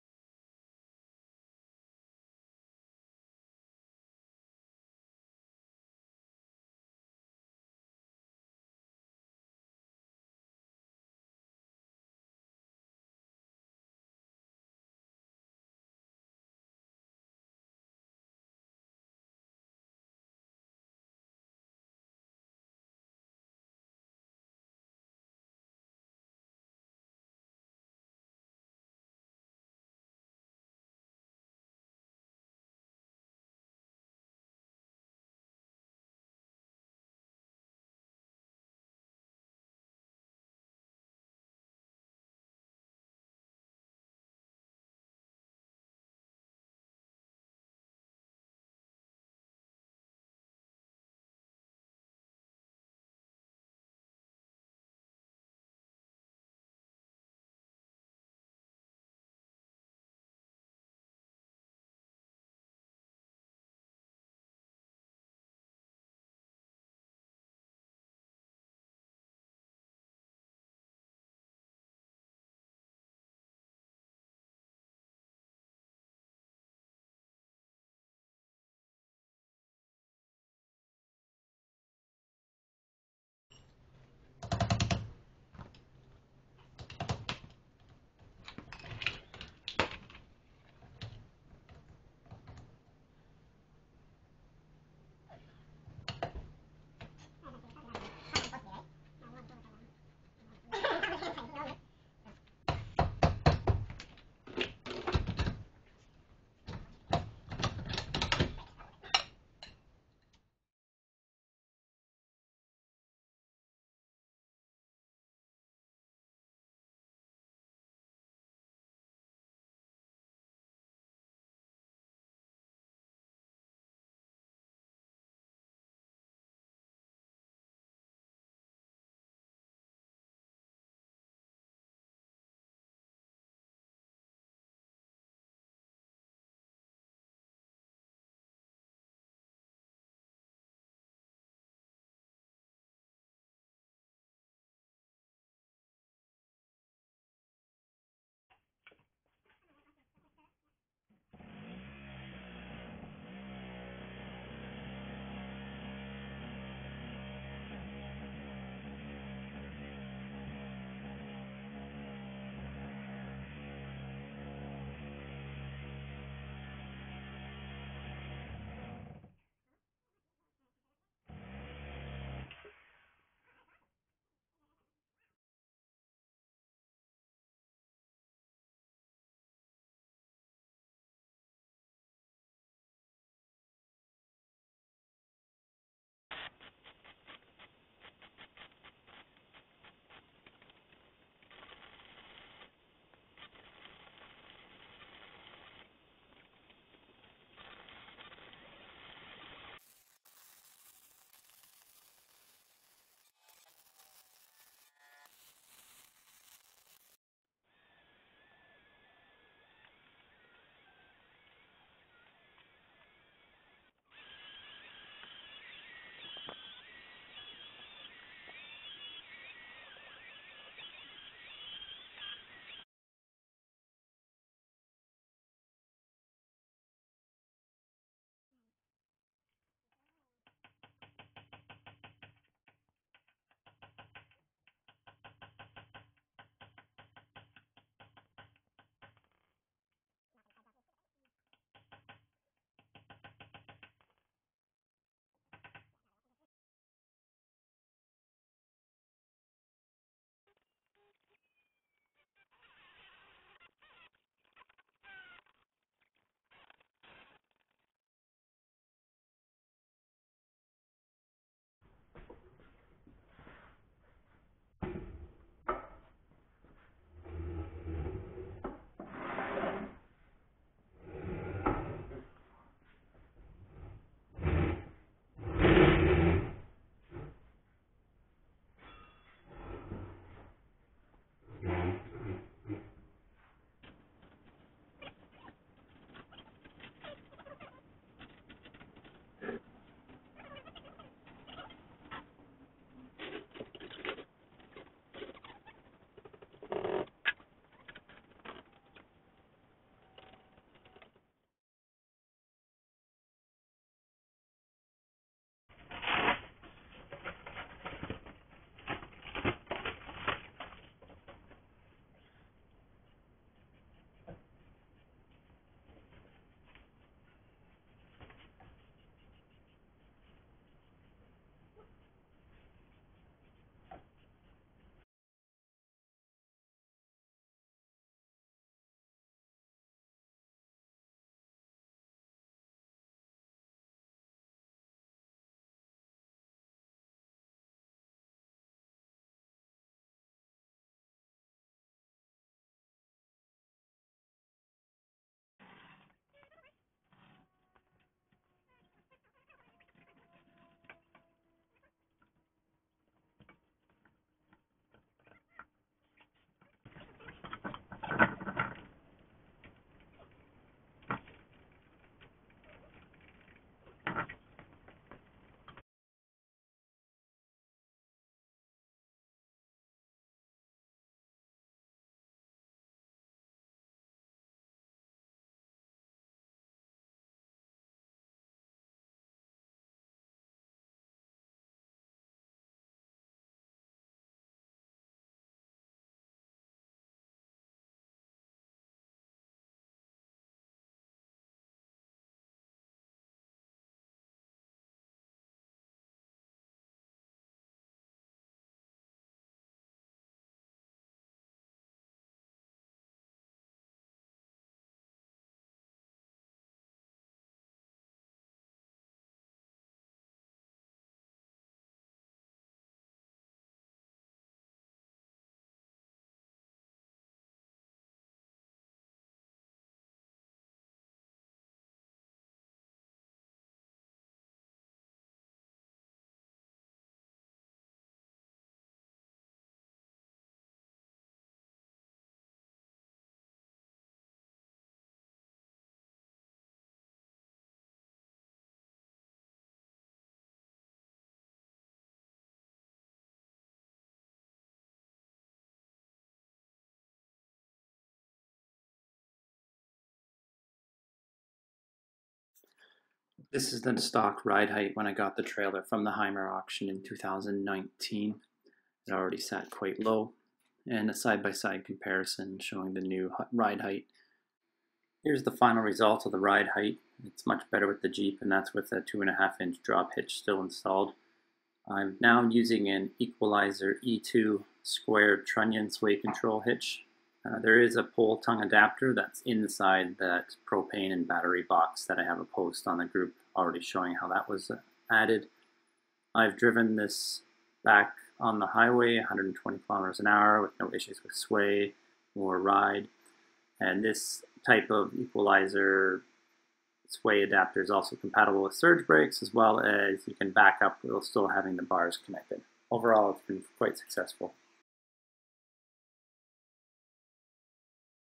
2019 Hymer Touring GT Axle lift/modification
This is how I modified my 2019 Hymer Touring GT to gain at least 5" of lift.

Parts
1. Lippert torsion axle (factory matching one)
2. 4x Grade 5 bolts
3. 8x flat washers
4. 8x lock washers
5. DOT3 Brake fluid (if working with hydraulic surge brakes)
6. Rust-Oleum Professional Cold Galvanizing Compound 568 G $11.97
7. Flat bar (to make 2 x 2.5”x2.5” square caps for the cut off ends)
8. Square U-bolts x4 (I got mine made at a spring shop at 2.75” inside spacing which I found was too loose. I’d get them narrower in the future)

Tools
- Floor jack(s)
- Jack stands
- Tape measure
- Combination square
- Pencil/marker
- Wood blocking (6x6 or 4x4)
- Sawzall (corded, not battery powered)
- Heavy duty metal carbide blade(s)
o Diablo 6 Inch Carbide Reciprocating Saw Blade Bonus 2-Pack ($14.97 (I used this type and only needed 1 blade)
OR
o Milwaukee Tool 9-Inch 7 TPI Torch Carbide Teeth Metal Cutting SAWZALL Blade ($21.78
- Alternatively, acetylene torch to cut off spindle arms (Sawzall method makes a cleaner cut)
- Angle grinder with cut-off discs, sanding/flex discs etc.
- Drill
- Metal drill bits (good quality and may need to be sharpened if dulled during process)
- Center punch
- Socket set, wrench set
- Hammer
- Flat head screw driver
- Bucket to catch brake fluid
- Brake bleed kit or tubing to connect to bleeder valve
- Welder (can be optional if you choose not to weld channel cut-out or end caps)